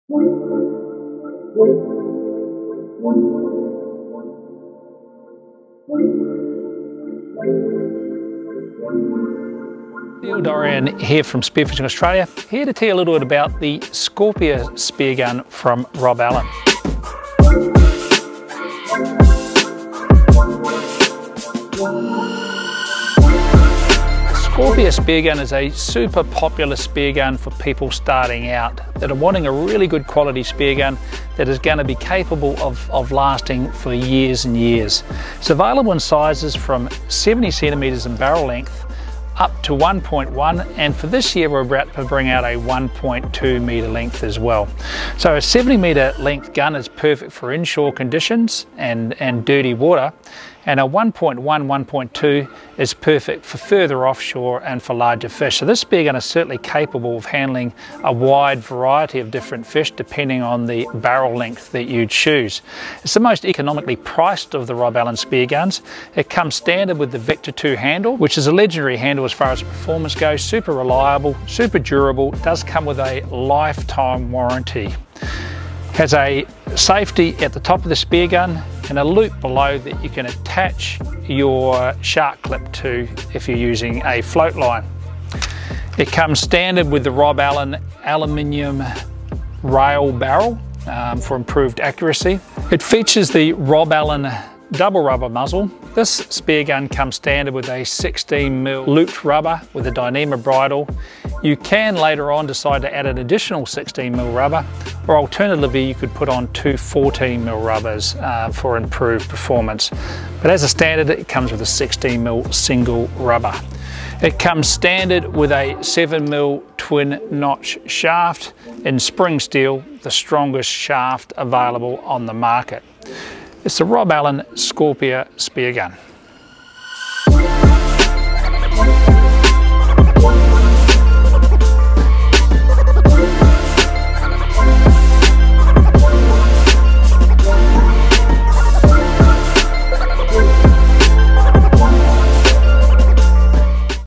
Rob Allen Scorpia Speargun
A cost effective gun that will still perform when confronted with a larger fish.
- Next generation Vecta 2 handle and mechanism
- Aluminium rail barrel made from aircraft grade aluminium
- Single rubber closed muzzle with 16mm rubber
- 7mm shaft made from oil quenched carbon alloy spring steel
- High strength, abrasion resistant black mono shooting line
- Available barrel lengths 700, 800, 900, 1000, 1100mm

More information available at Spearfishingaustralia.com